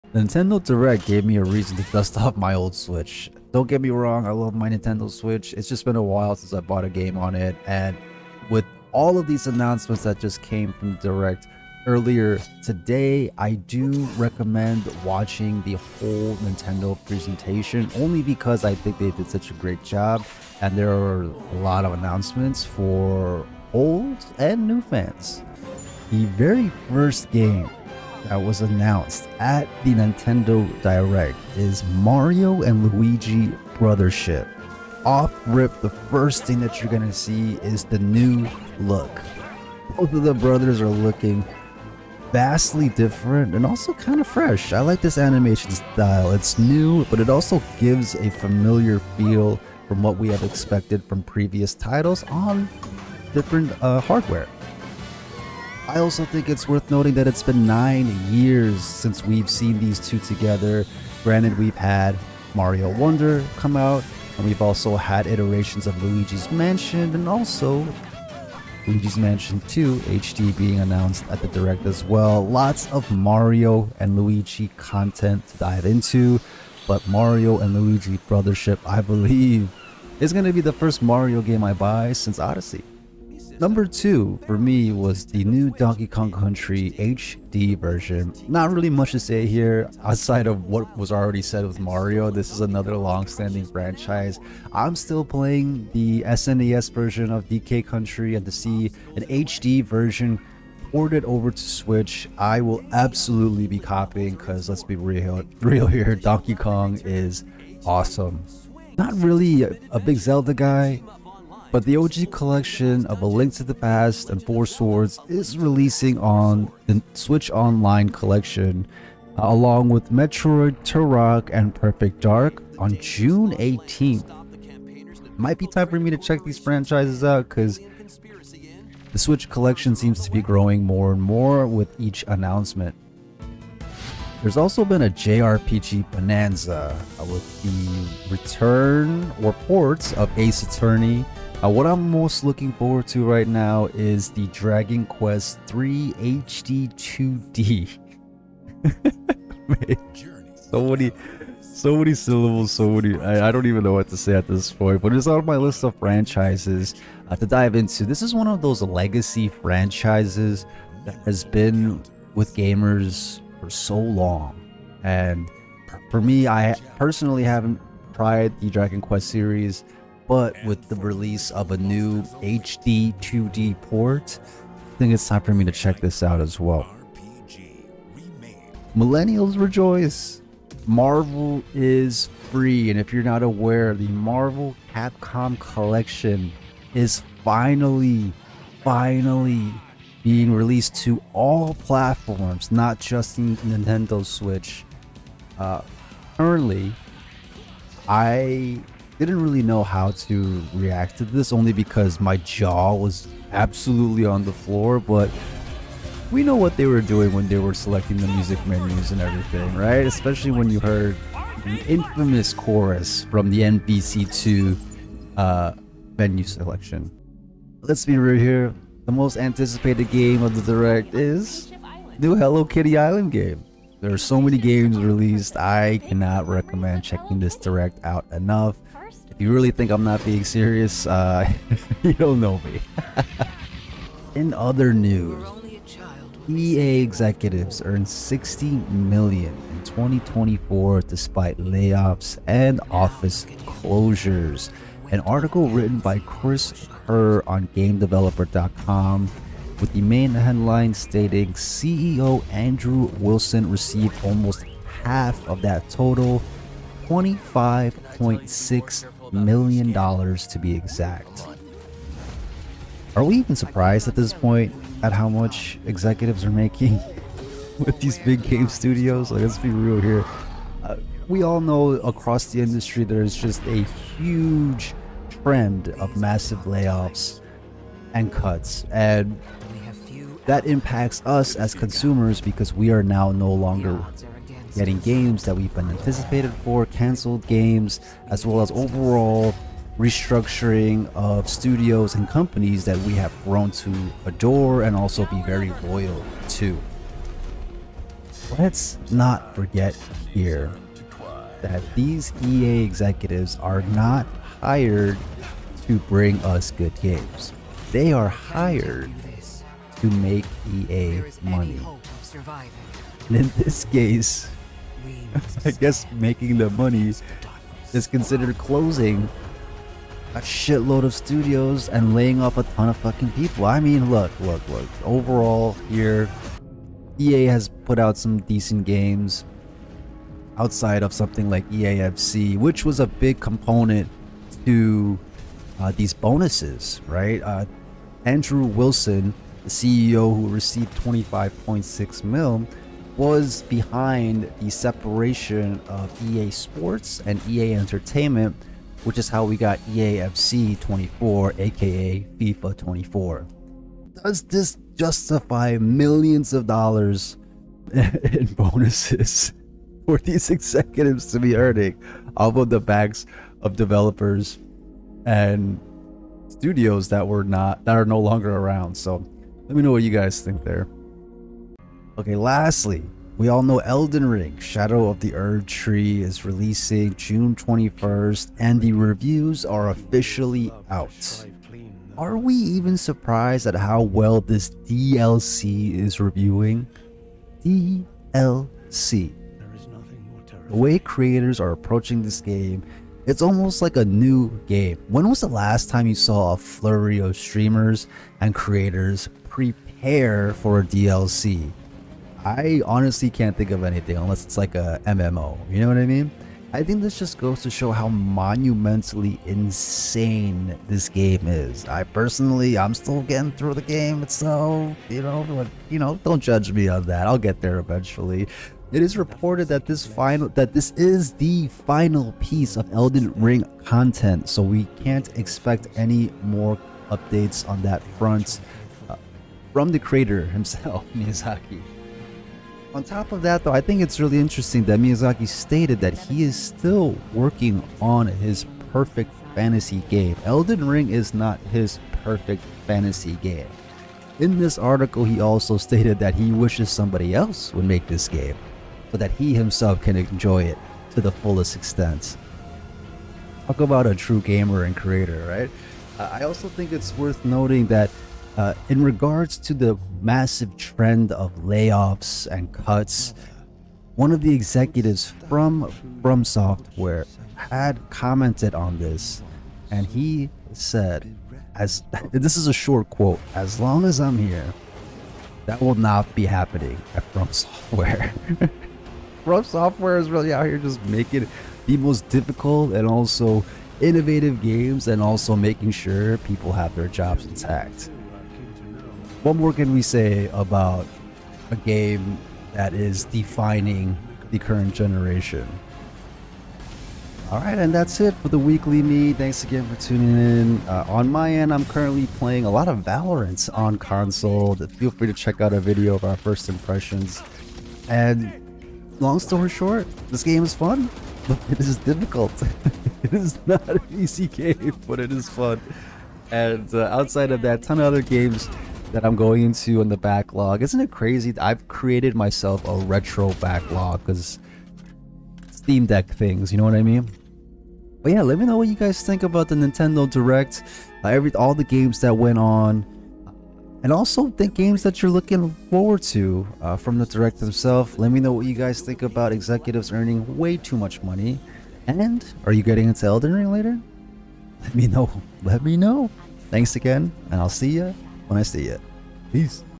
Nintendo Direct, Elden Ring DLC Reviews, FromSoftware on layoffs in Gaming
This week on The Mead -  Nintendo Direct was a solid showcase. A few of my favorite announcements highlighted and I recommend watching the Direct. EA executives profit amid massive layoffs. Elden Ring Shadow of the Erdtree creator talks game taste. FromSoftware speaks on the massive layoffs and cuts among larger game studios.

What a start to the summer! For more game news and commentary, like and sub. Thanks for tuning in, your mead has been served ☕️

0:00 Intro
0:26 Nintendo Direct
3:55 EA Executives Rake it in
6:15 Elden Ring DLC/FromSoftware talks layoffs
8:21 Closing thoughts and impressions

Next Stream: 🔴Live 6/19 530PM PST